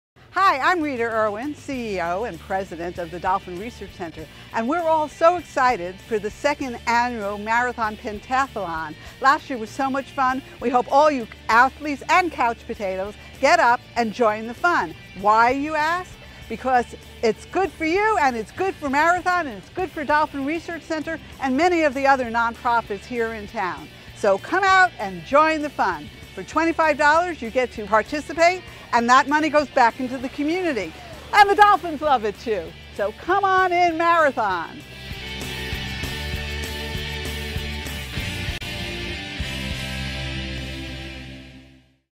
Dolphin Research Center is a non-profit who benefits from The Marathon Games
Dolphin Research Center is a non-profit benefiting from The Marathon Games October 7, 2017 at The Florida Keys Country Club. for more info.